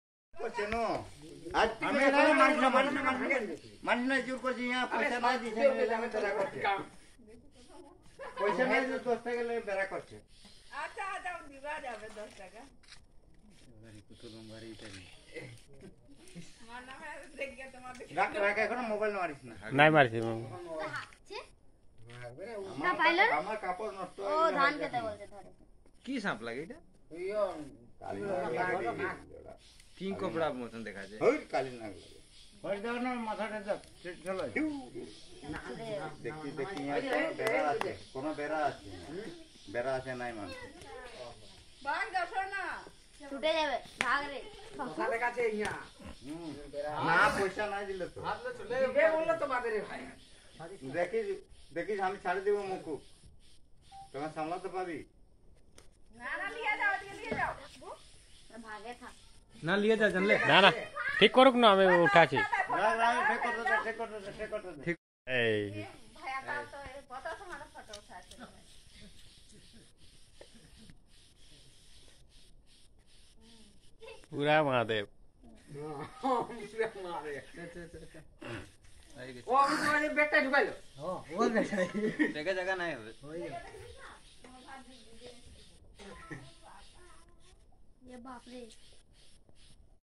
Cachar Man bitten to death by King Cobra
The man held the snake and was playing with it. The snake ended up biting the man to death